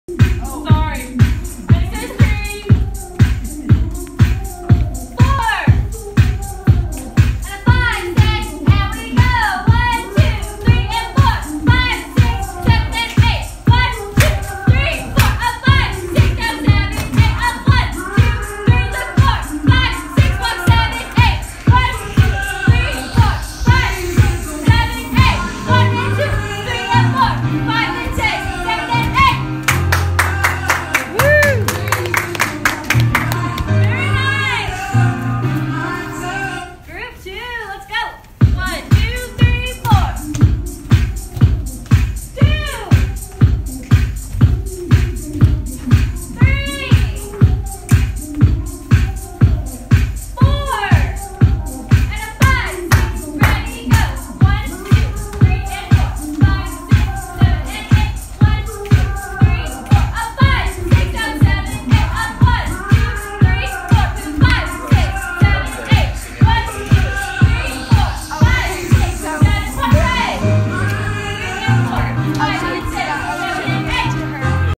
Intermediate/Advanced Jazz- group 1
Jazz combo with Miss Stormi Luney during our Intermediate/Advanced Day of Dance!